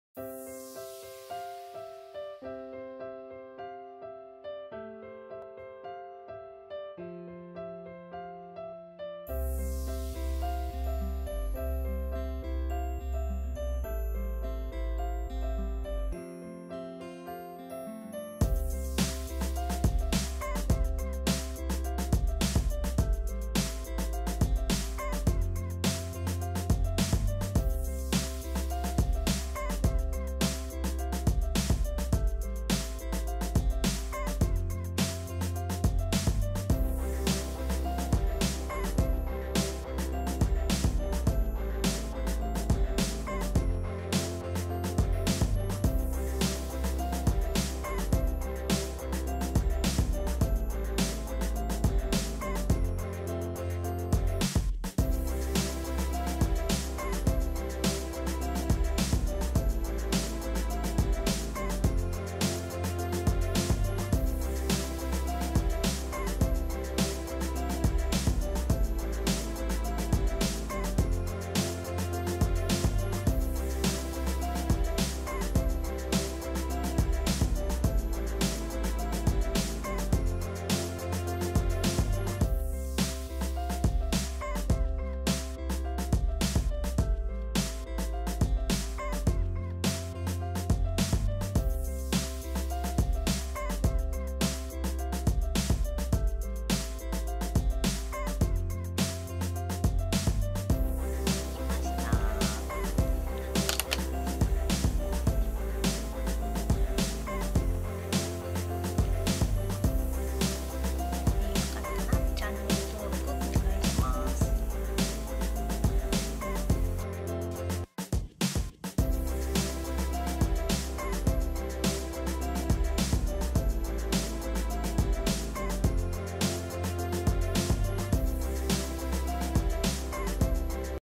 snowのフィルターみたいなアニマルメイク！うさぎのフェイスペイント
snowのフィルターみたいなうさぎのアートメイクです！アニマルメイクでうさぎのフェイスペイントの描き方を紹介します。
ボディペイントアーティスト　Body paint　artist

あったらいいなをえがきます
えがき屋　都愛ともか